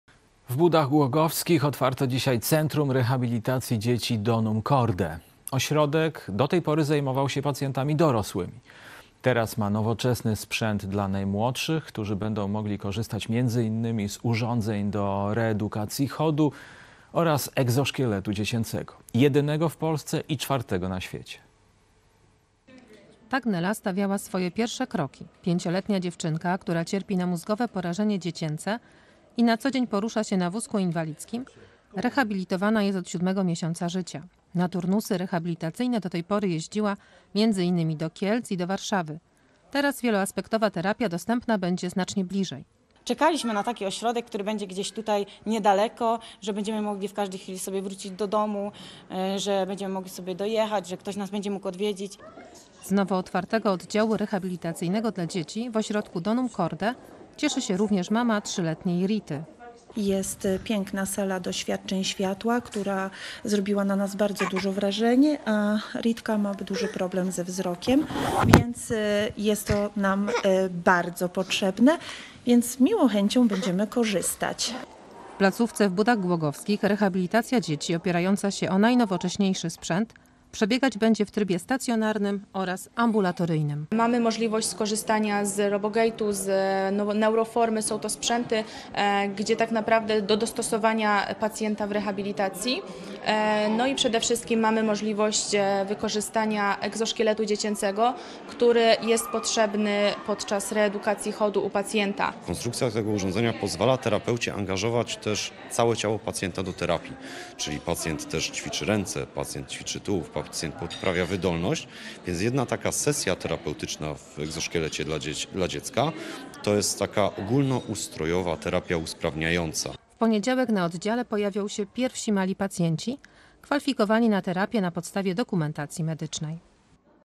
Centrum Rehabilitacji Dzieci DONUM CORDE
Raz jeszcze wracamy wspomnieniem do otwarcia oddziału CENTRUM REHABILITACJI DZIECI Donum Corde 💚
My już działamy i intensywnie pracujemy and sprawnością naszych młodszych Pacjentów! 🤗
Dziękujemy TVP3 Rzeszów za objęcie nas patronatem medialnym! 😊